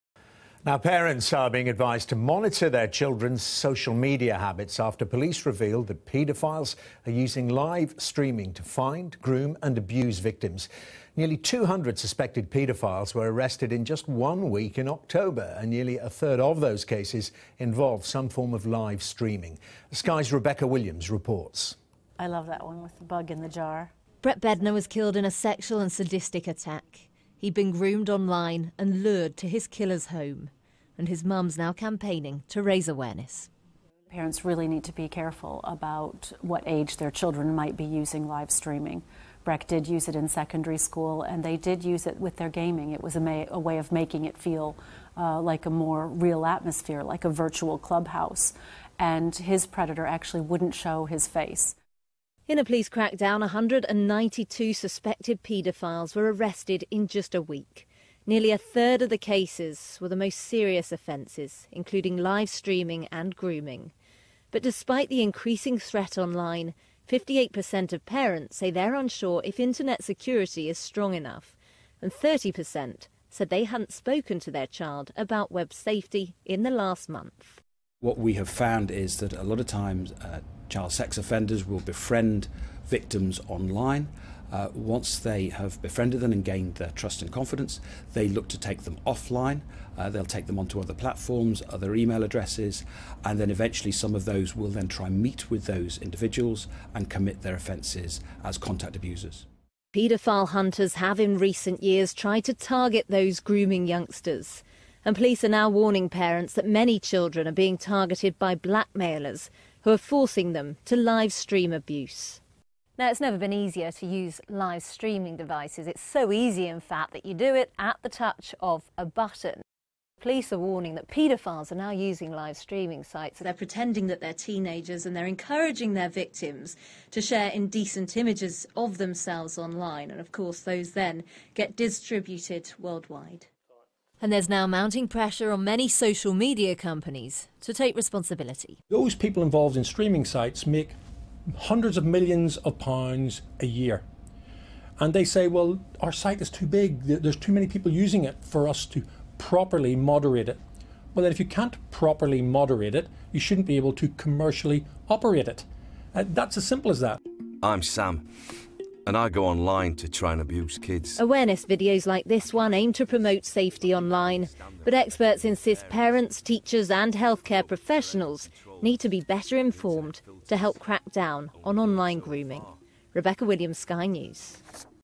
Live streaming used to groom children - Rebecca Williams reports
Suspects are using live streaming with dares, threats or promises of rewards to convince children to take their clothes off.

Police say paedophiles are using live streaming to bombard targets with comments, dares, threats or to offer rewards with the aim of convincing them to take their clothes off on a webcam.

Rebecca Williams reports.